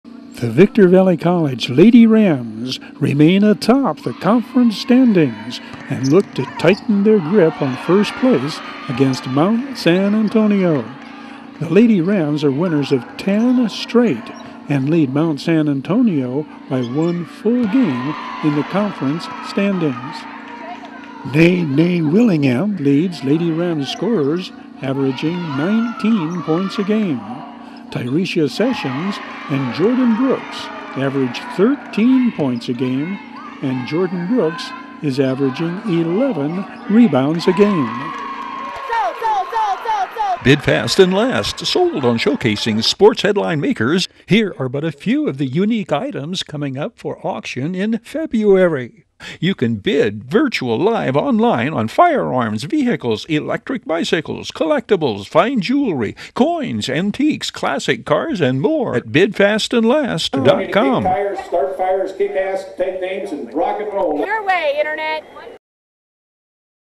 VVC LADY RAMS ATOP CONFERENCE
The Victor Valley College Lady Rams remain atop the conference standings, and look to tighten their grip on first place against Mount San Antonio.